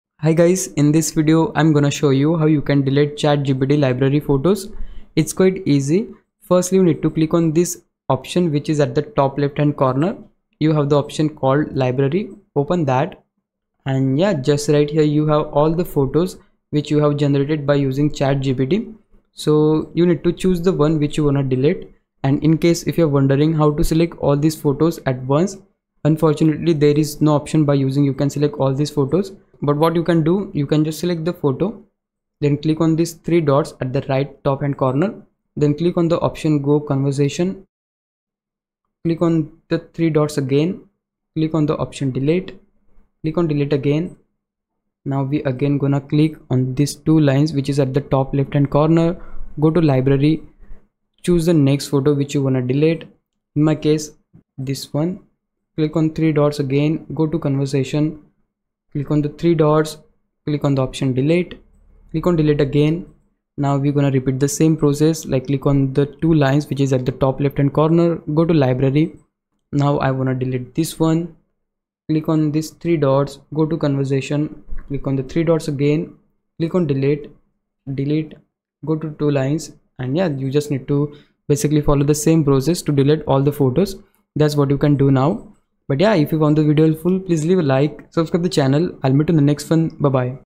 How to Delete ChatGPT Library Photos (EASY)
In this video, I show you how to delete chatgpt library photos. Did this video help? Thanks!